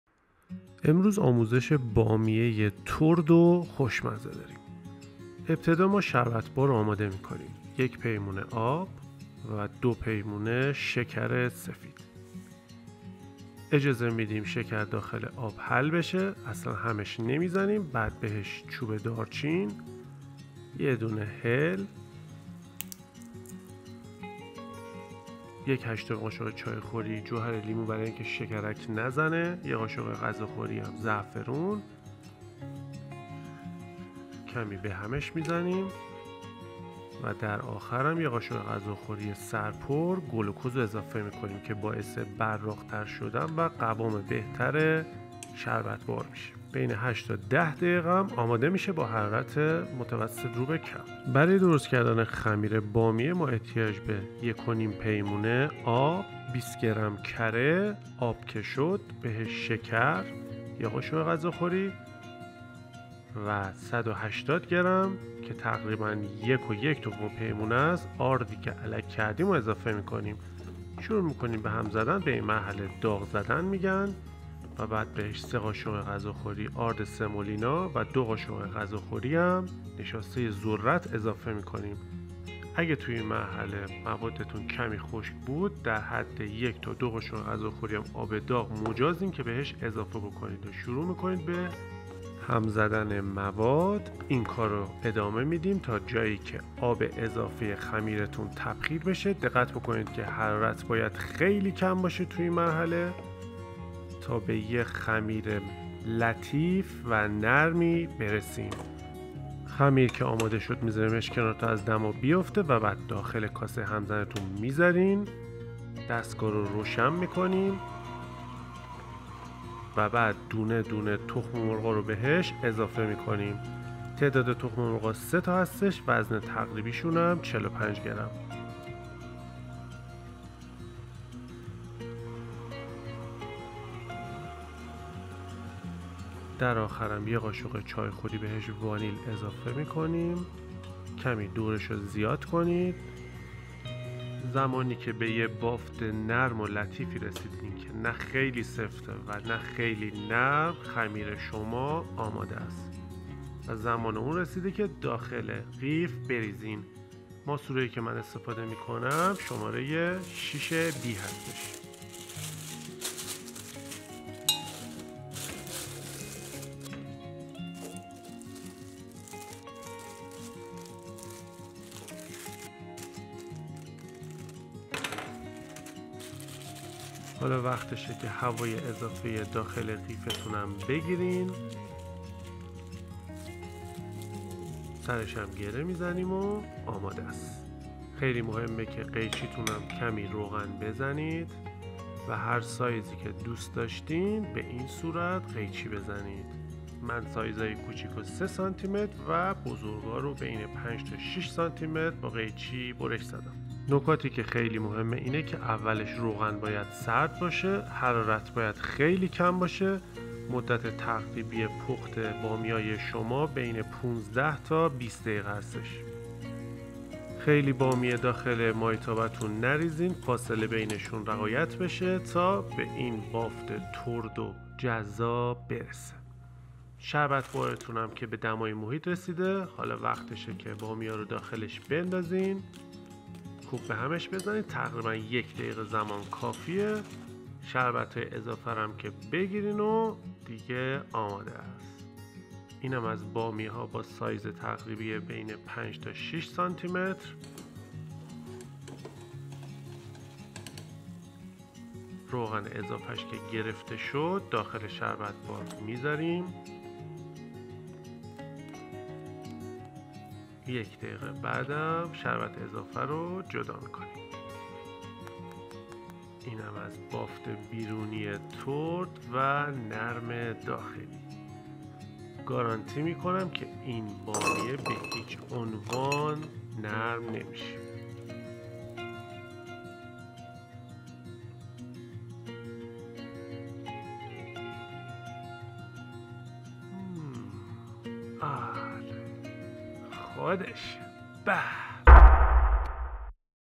شیرینی بامیه اصل بازاری با تمام نکات
امروز آموزش شیرینی بامیه داریم بسیار ترد و خوشمزه که بعد از چند ساعت به هیچ عنوان نرم نمیشه .
مواد لازم :
آب  یک و یک دوم پیمانه ۳۰۰ گرم
کره ۲ ق.غ یا ۲۰ گرم
شکر ۱ ق.غ یا ۱۵ گرم
آرد  ۳ صفر   ۱۸۰ گرم یا یک و یک دوم پیمانه
(هر پیمانه ارد و ۱۲۵ گرم‌در نظر بگیرین )
نشاسته ذرت ۲ ق.غ یا ۲۰ گرم
ارد سمولینا ۳ ق.غ یا ۳۰ گرم
تخم مرغ کوچک ۳ عدد هر کدام ۴۵ گرم
شربت بار :
آب ۱ پیمانه یا ۲۰۰ گرم
شکر ۲ پیمانه یا ۴۰۰ گرم
۱ ق.غ زعفران دم کرده غلیظ
هل و دارچین
گلوکز  ۱ ق.غ  سر پر
🔥گلوکز واجبه برای براق شدن بامیه ها و قوام بهتر شربت .
1️⃣آرد سمولینا لازمه برای تردی بافت بامیه ها.
2️⃣نشاسته ضروریه برای بافت خمیر .
3️⃣شربت بار باید  ۱۰ دقیقه با حرارت کم پیجوشه تا حباب های ریز پر بشه و غلظتش  شبیه عسل روان بشه .
4️⃣حتما تخم مرغ کوچک باشه وزن تقریبی ۴۵ گرم .
 5️⃣تعداد بامیه هایی که داخل روغن میندازیم نباید زیاد باشه یعنی تابه نباید پر باشه و بین بامیه ها فاصله باشه چون بخار بامیه ها باعث میشه ترم بشن.
6️⃣ بعد پخت بامیه حدود هفت الی هشت ساعت روش رو نپوشونید و بدون رو کش داخل یخچال بزارین تا نرم نشن. هر بار که خواستین بامیه رو داخل روغن بریزید از روی حرارت بردارین.
 7️⃣مهمترین قسمت بامیه داغ زدن هست که همون پختن خمیر روی حرارت که باید با شعله کم پخته بشه وآب درون خمیر تبخیر بشه.
8️⃣ شهد هم درکیفیت بامیه تاثیر زیادی داره اگر به خوبی قوام نیومده باشه بامیه نرم میشه اگر هم زیادی قوام بیاد بامیه شکرک میزنه.
9️⃣ بامیه های که بعد از چندساعت نرم میشن به خوبی رو گاز پخته نشدن و یا شهد بخوبی قوام نیومده و یا با شعله بالا سرخ کردند!